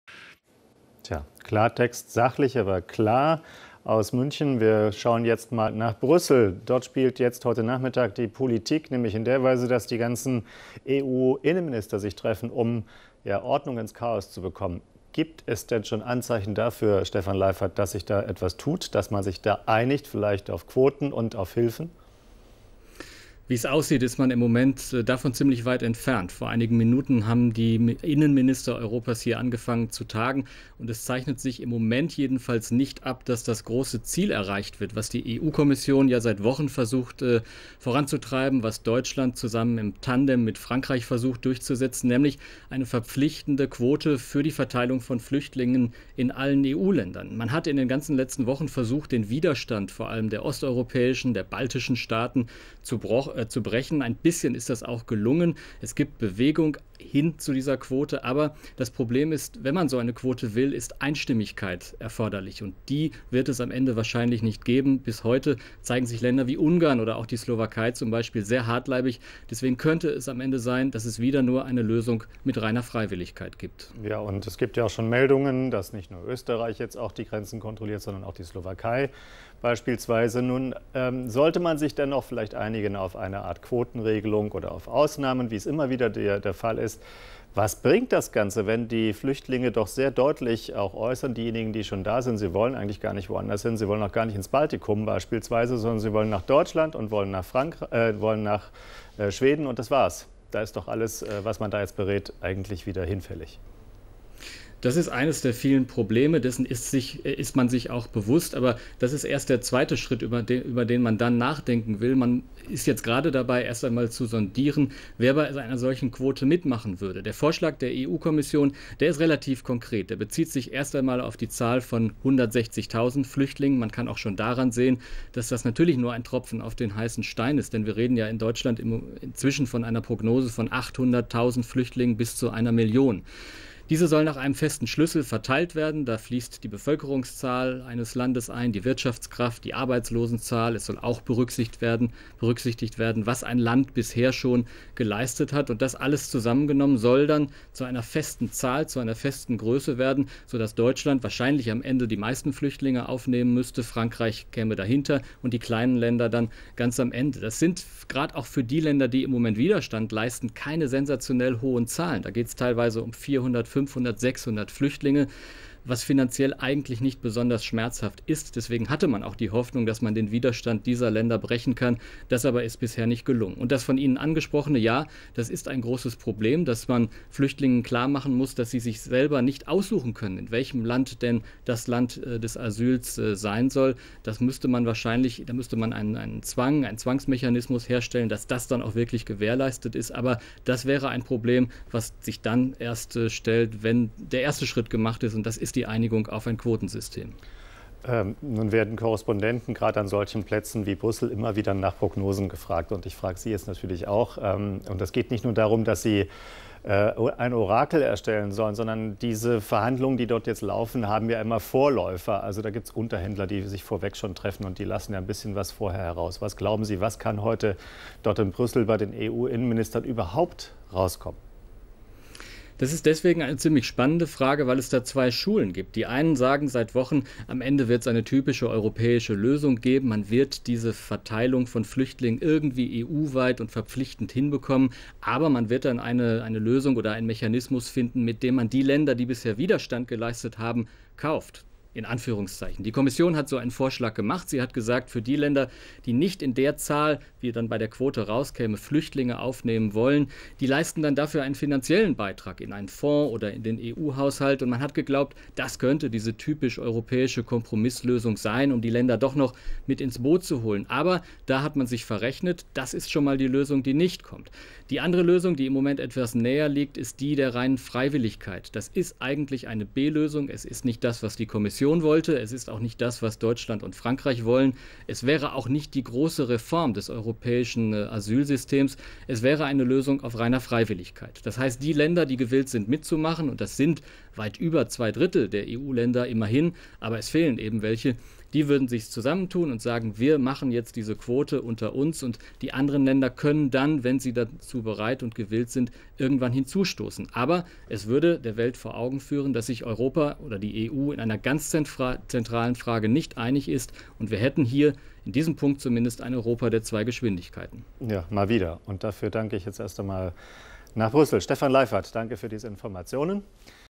Flüchtlingskrise: Stefan Leifert zum Treffen der EU-Justiz- und Innenminister am 14.09.2015
Stephan Kulle im Schaltgespräch mit phoenix-Korrespondent Stefan Leifert zum Treffen der EU-Justiz- und Innenminister in Brüssel und zur europäischen Asylpolitik.